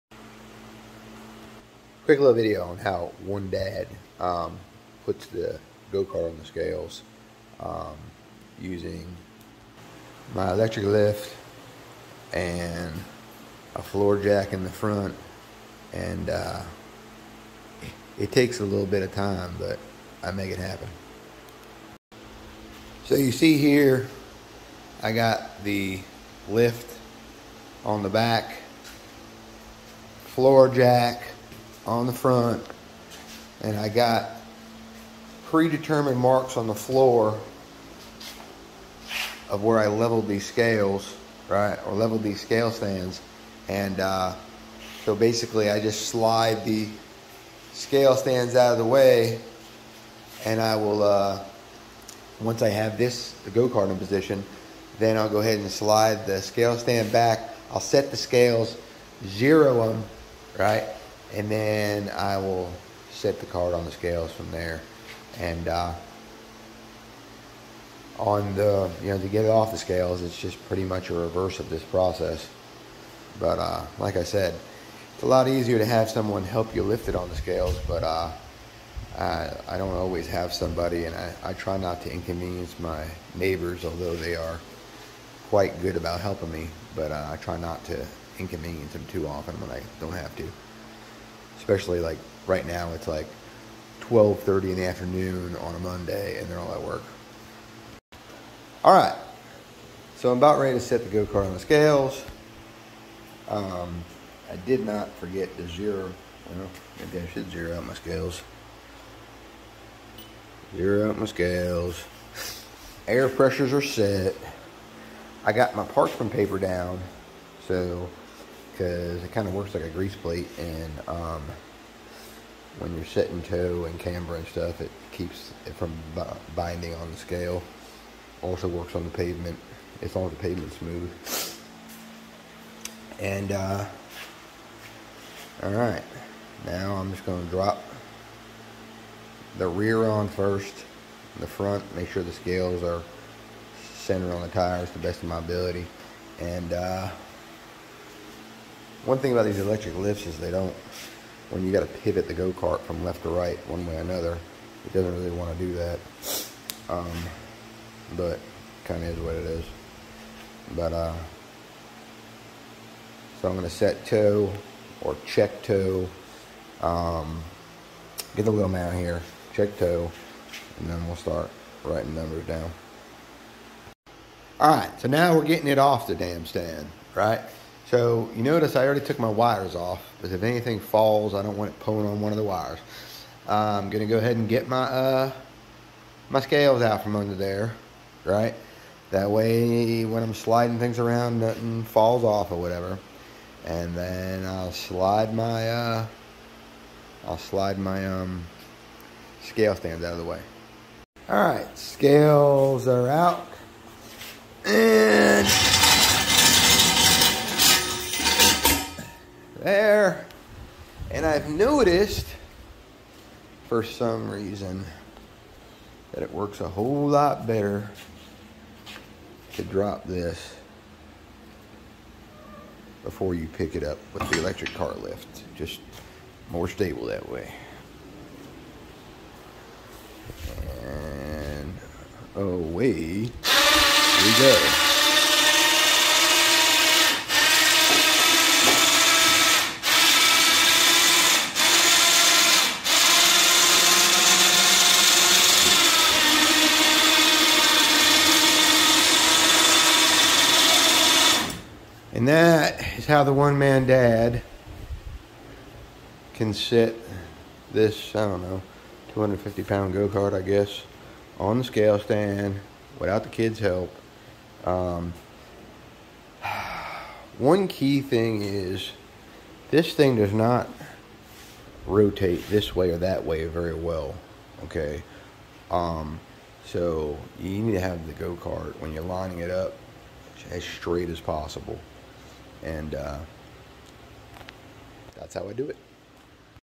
How this Kart Dad gets our Wing Kart on the Scales by myself.
Fact is this karts are heavy, and the kids just can't help lift them. Some tiems you have to get creative. At some point I might create a pulley system in the garage. But for now. This is how I do it.